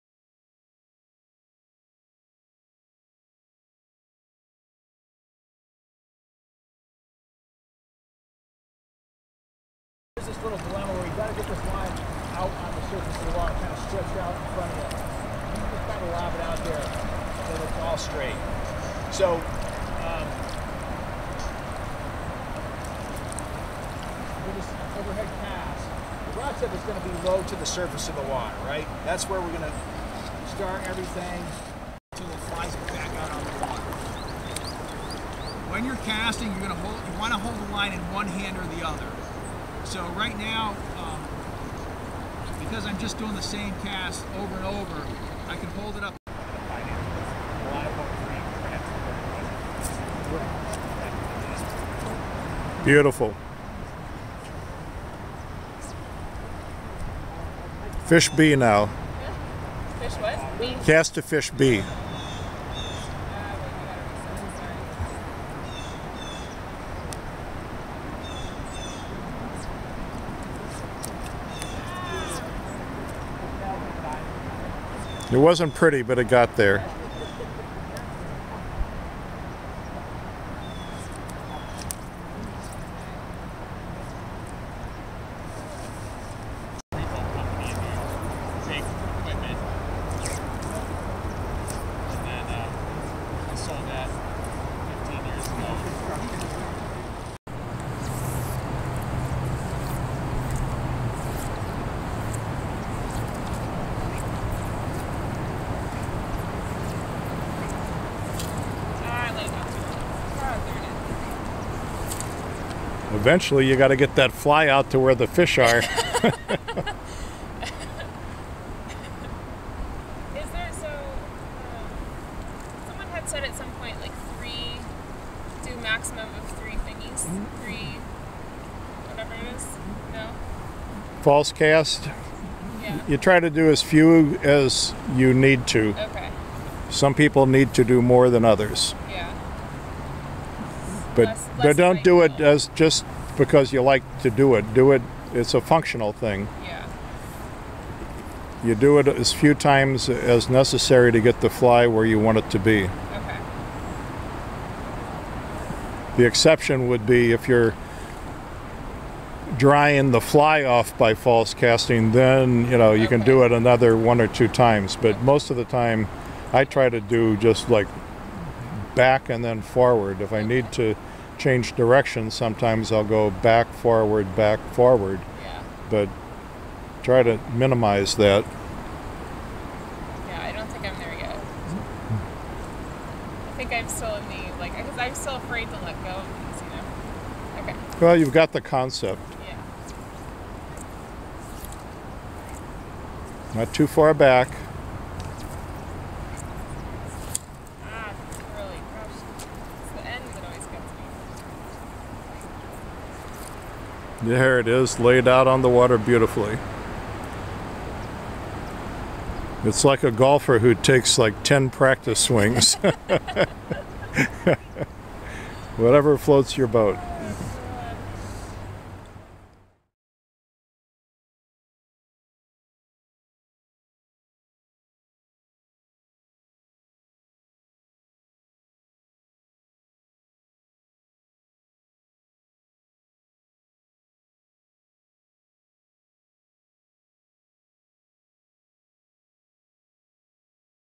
Learn to Fly Fish NH
The October class of the Northeast Fly Fishing School learned to Think like a fish & act like a bug.  The on-the-water lesson taught the new fly anglers the roll cast, the overhead cast, how to fish a dry fly, nymph and streamer. Knot tying and equipment overview were also covered.